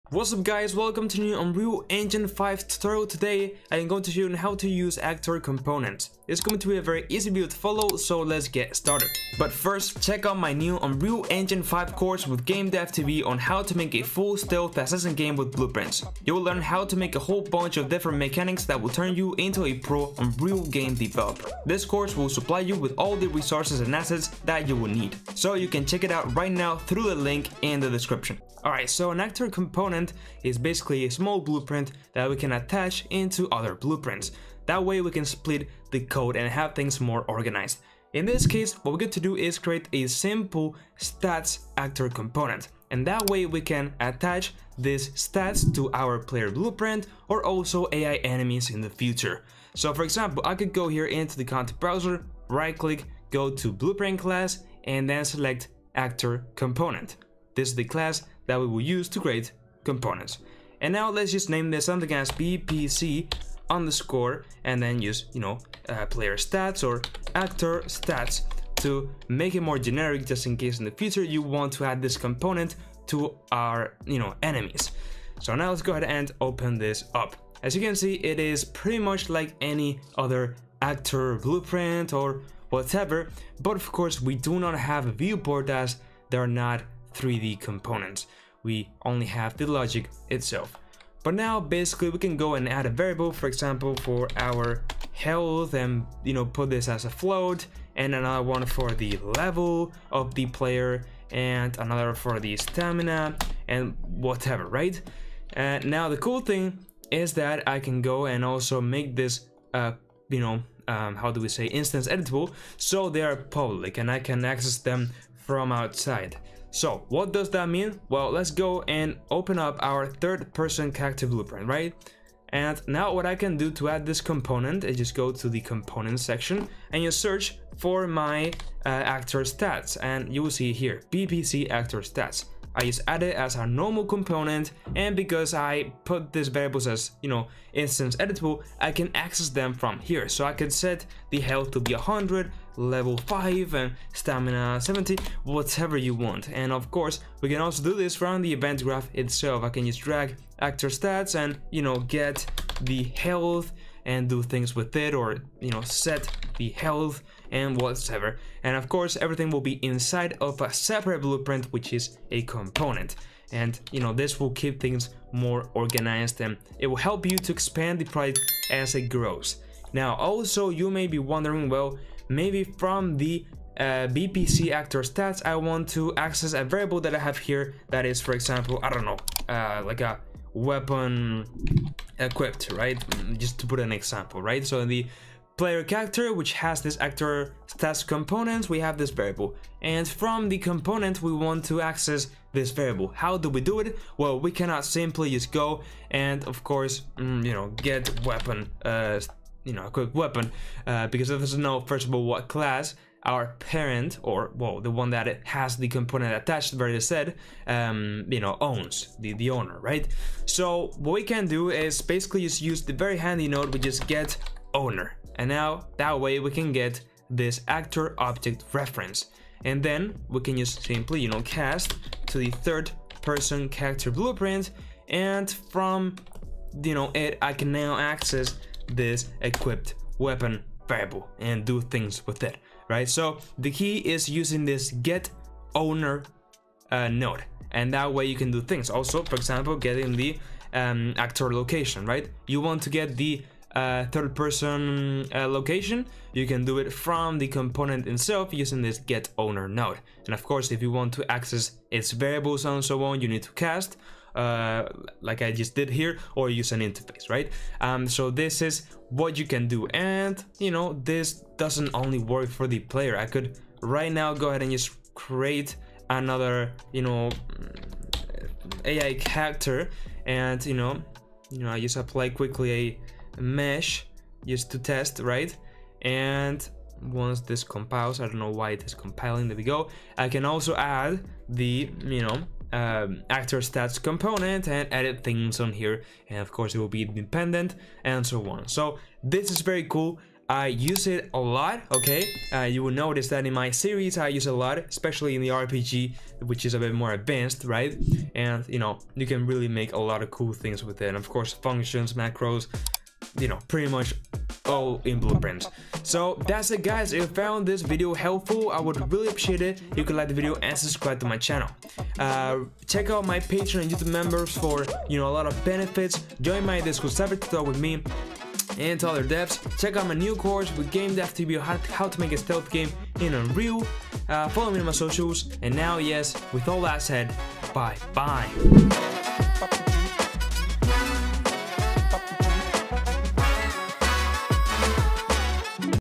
How to Use Actor Components in Unreal Engine 5 - Beginner Tutorial
Hello guys, in this quick and simple tutorial we are going to see how we can use actor components in Unreal Engine 5.